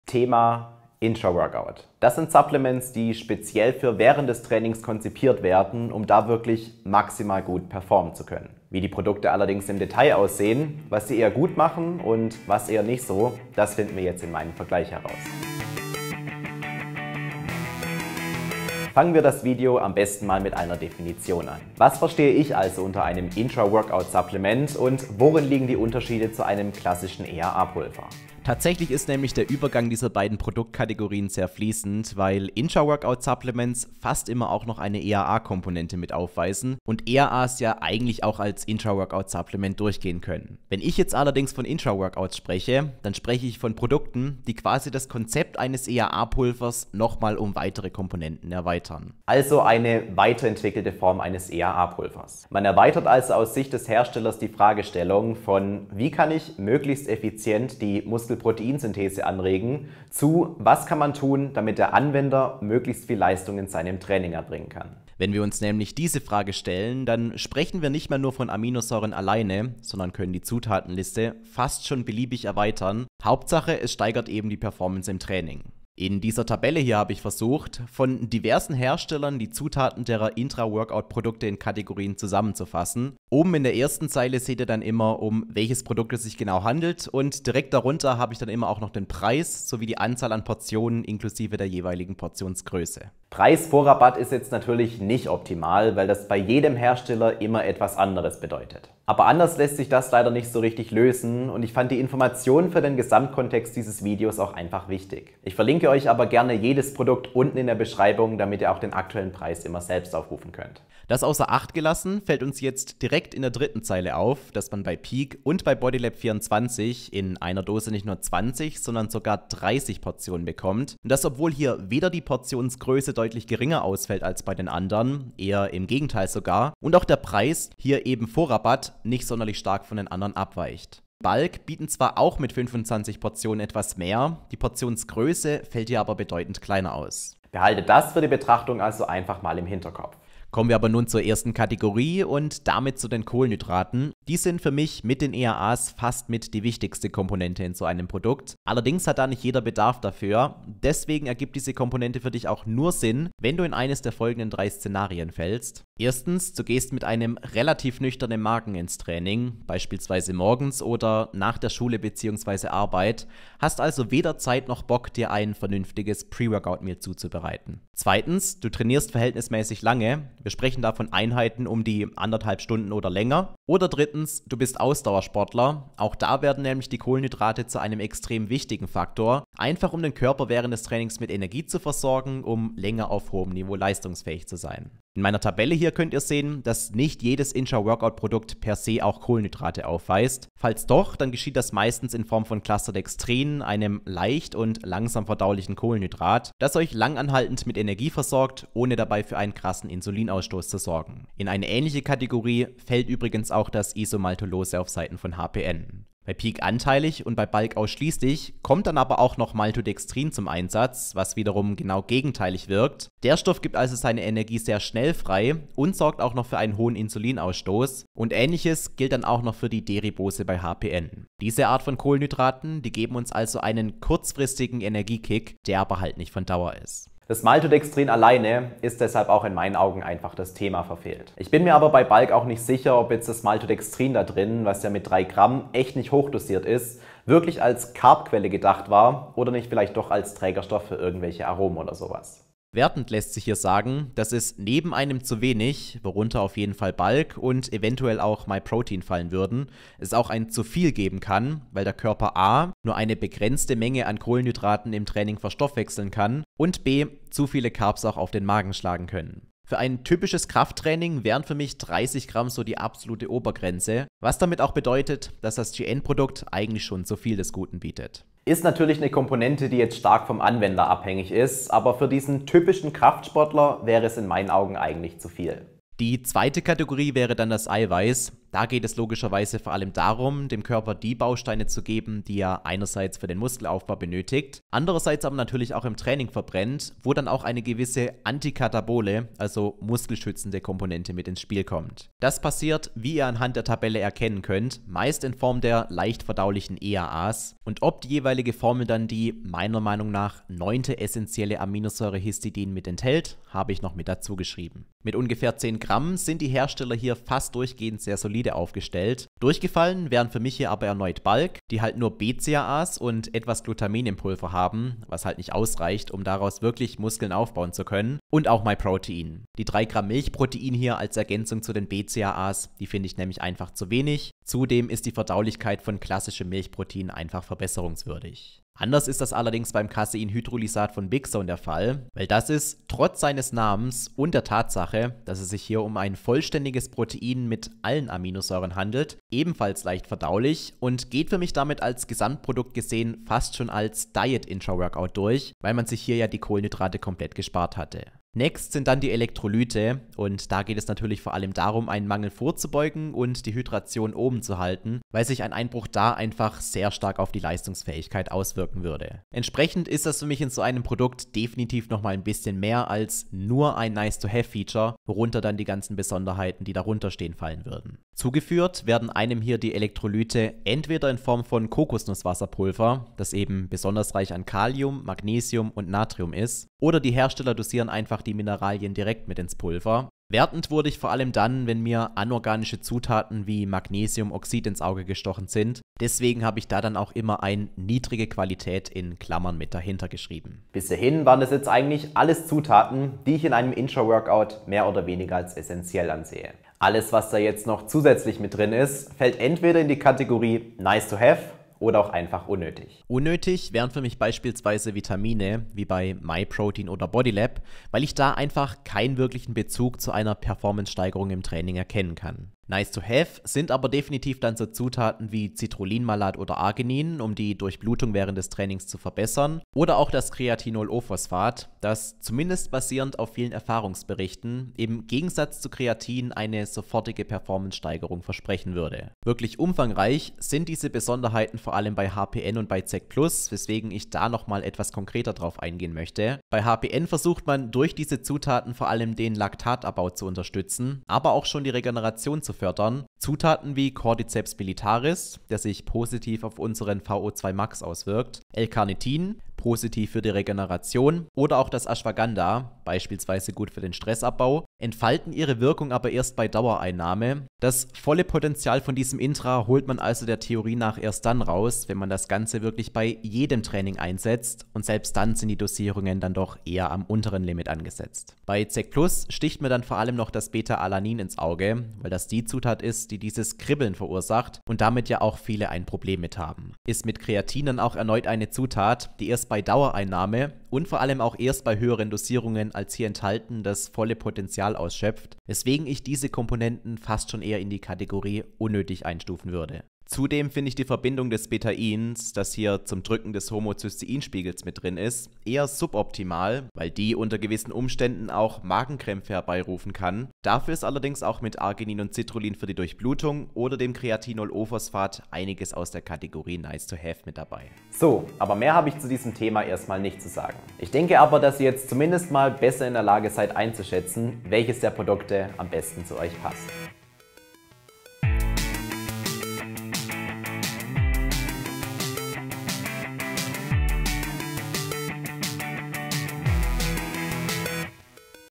Ein Intra Workout Supplement Vergleich | Fitness Food Corner
Es ist eine Weile her, dass ich mich auf diese Weise einer Produktkategorie genähert habe, aber bei den Intra Workout Supplements ergibt es einfach Sinn, die Zutatenlisten mehr oder weniger direkt gegenüber zustellen.

Welche Stärken und Schwächen die Pulver untereinander alle aufweisen, seht ihr im Video. Nachfolgend seht ihr auf jeden Fall nochmal alle Kandidaten:

👉 Immer den besten Preis mit dem Code "Felix"

*Unbezahlte Werbung wegen Produkterkennung.*

*Vielen Dank an meine Partner, die mir Produkte und Informationen für solche Videos bereitstellen.*